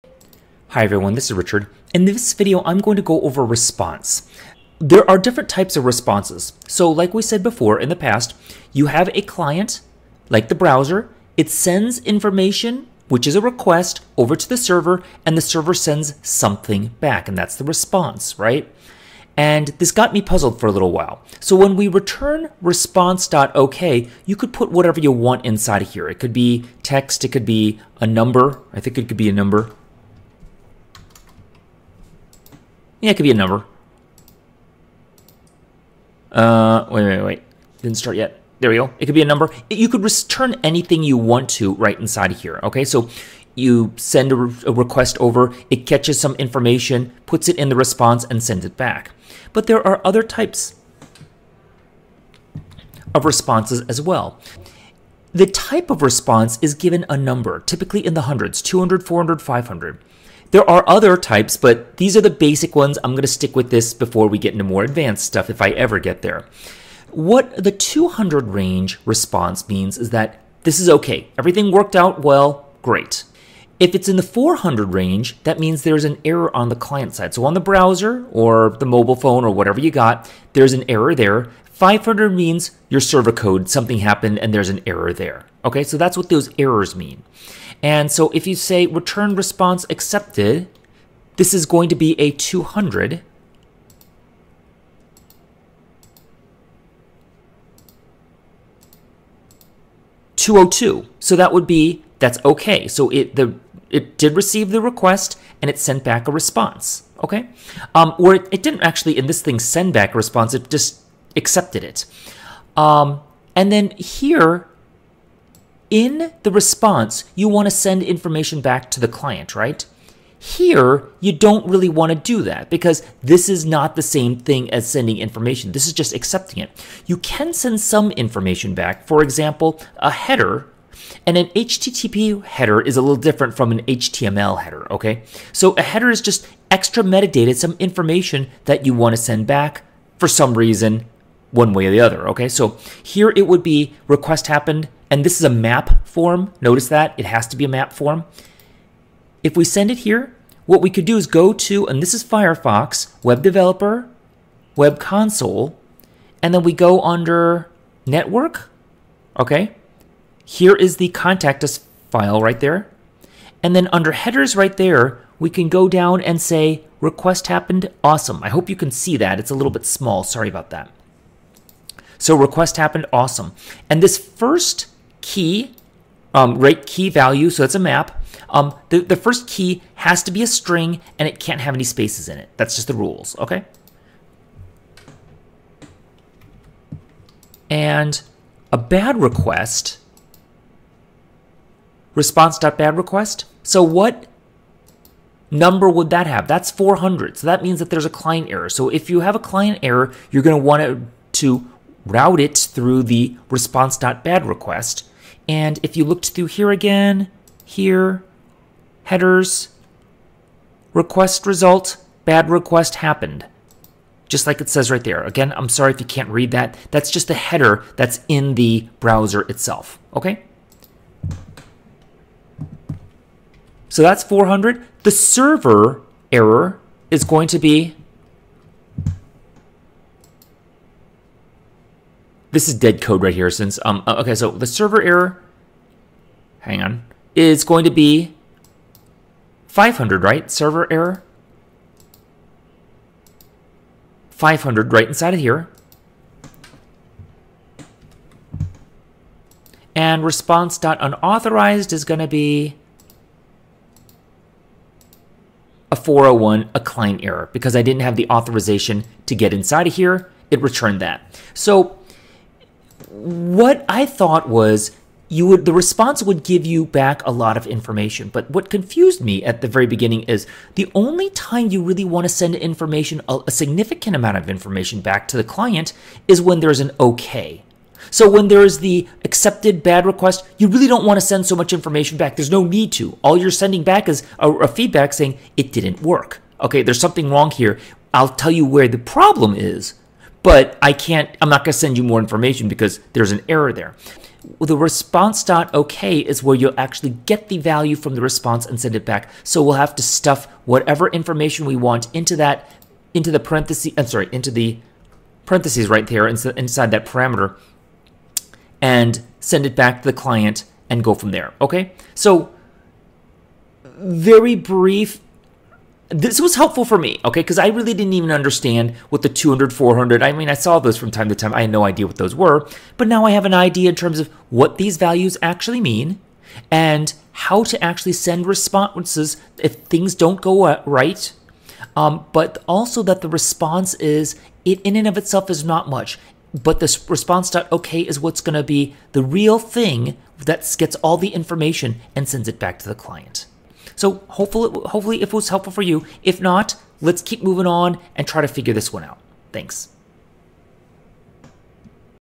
Server Side Dart: 08 Response
This is my experience learning the Dart Language on the server.  I have no previous experience with server side programming, so this is my journey so far.

Reviewing response seems premature, since it is typically at the end of the server function, but it seemed to be a good idea to get the basics first.